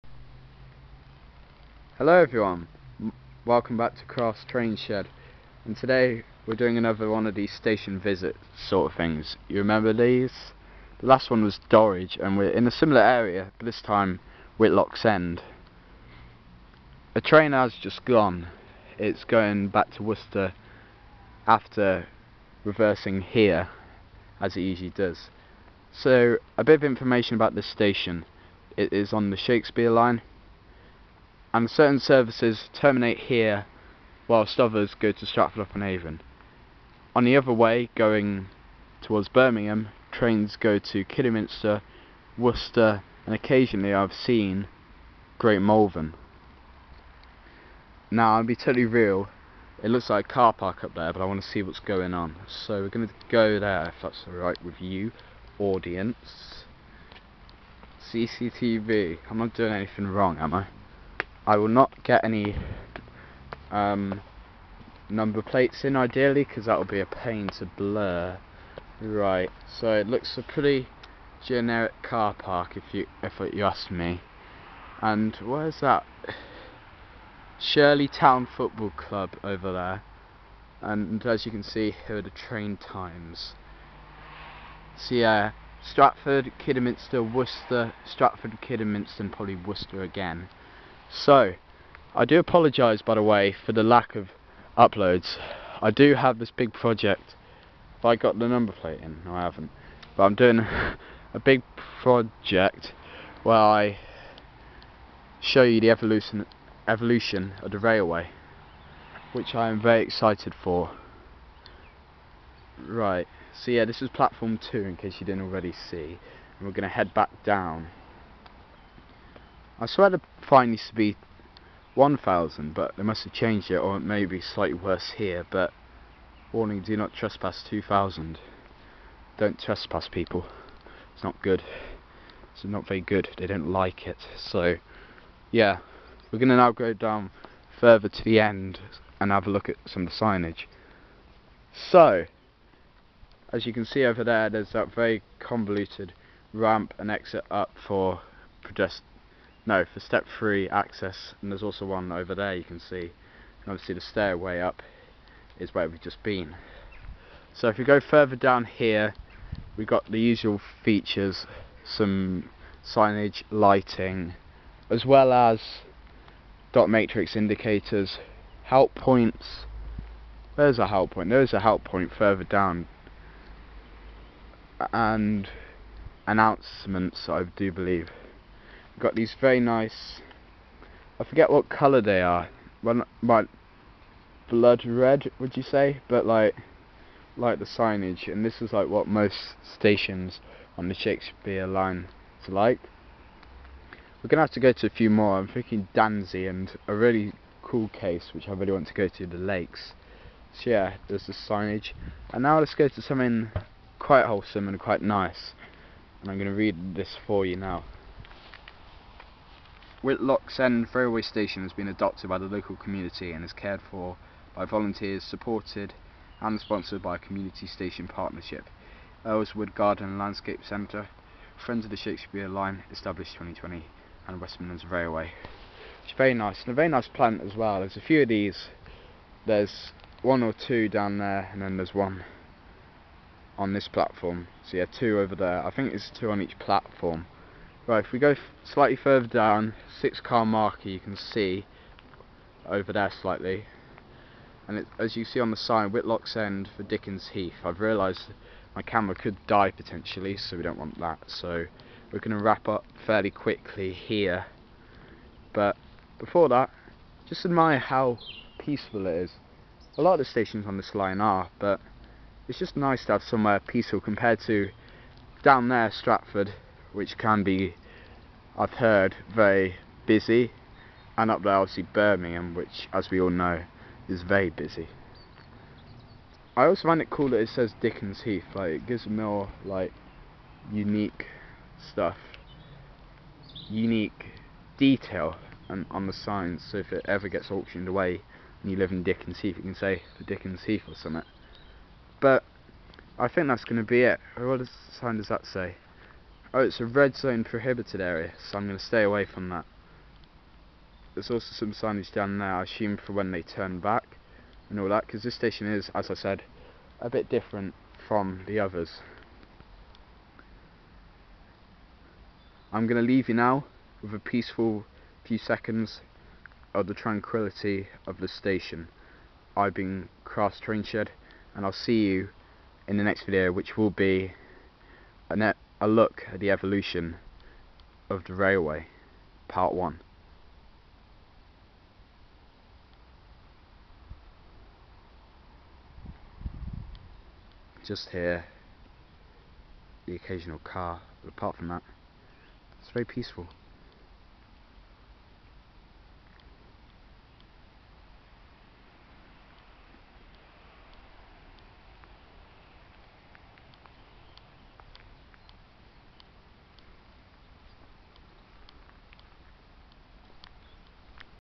Station Visits: Whitlocks End
Episode 4 of Station Visits
This time we return to the Midlands and this time Whitlocks End on the Shakespeare Line between Birmingham Moor Street and Stratford-Upon-Avon